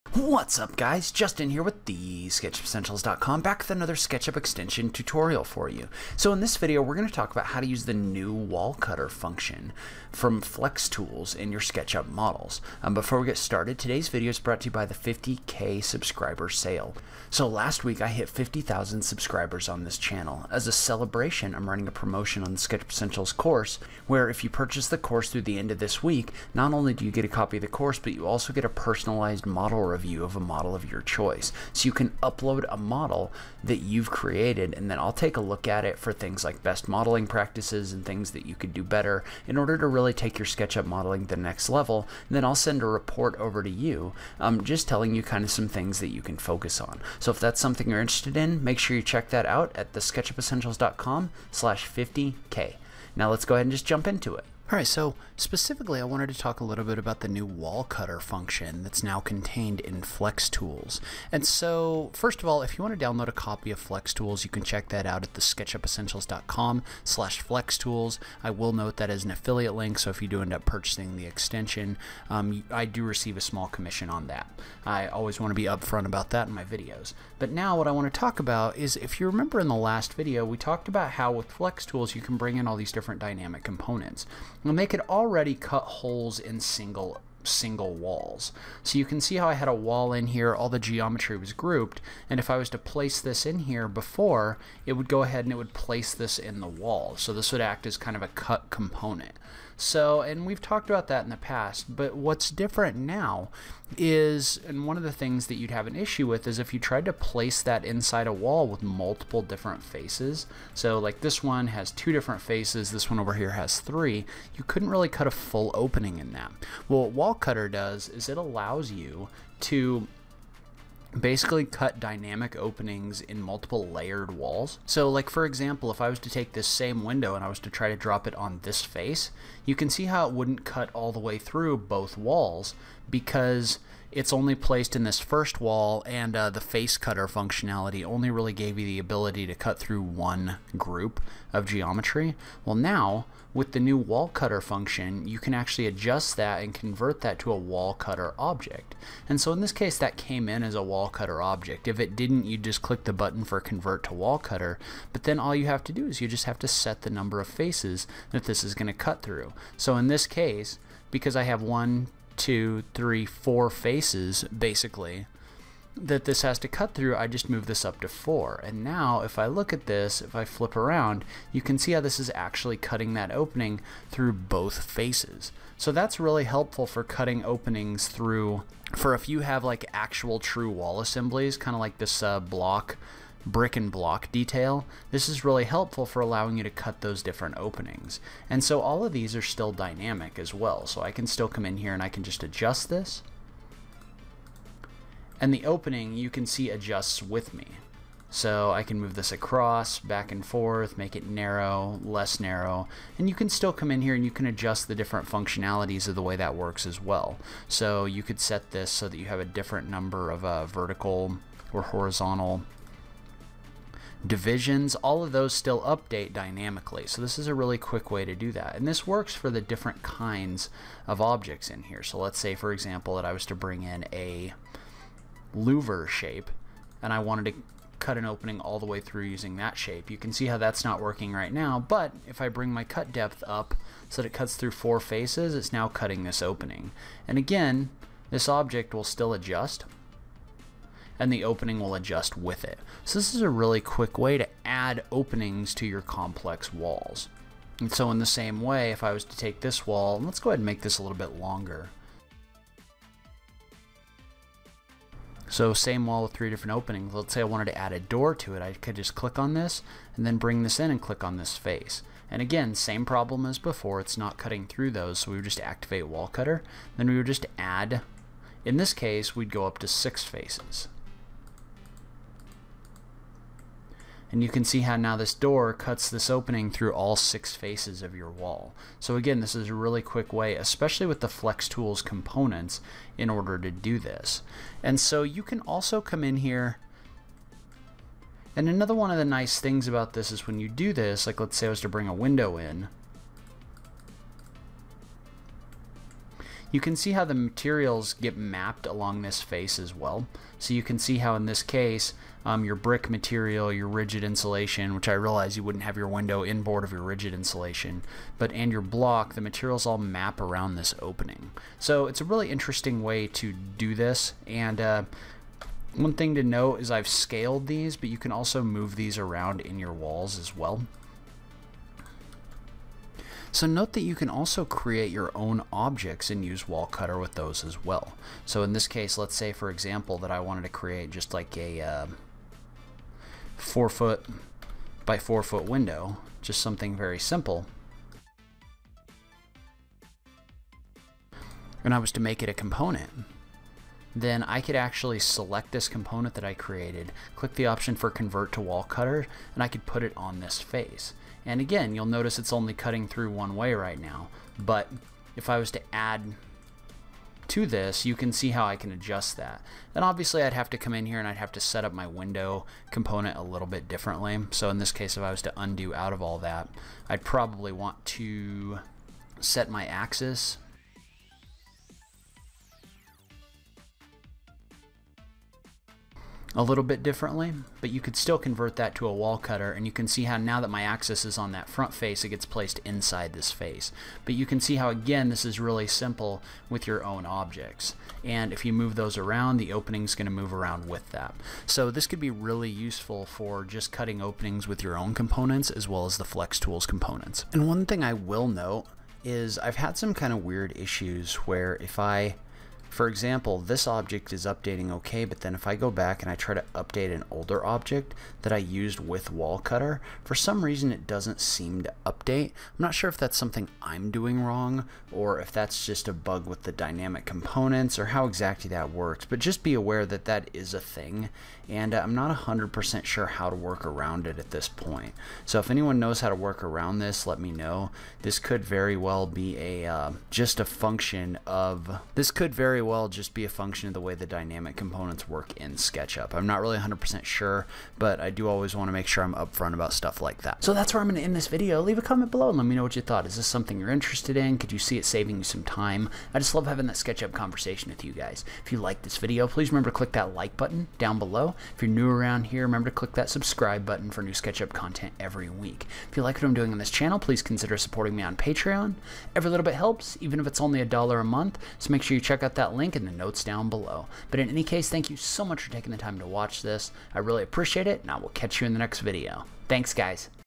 FlexTools Tutorial - Using Wall Cutter to Cut Holes in SketchUp Walls
In today's video, we check out a new feature contained within FlexTools for SketchUp - Wall Cutter! This tool allows you to cut holes for objects through multiple different layers of walls - it's great for placing windows and doors in your models!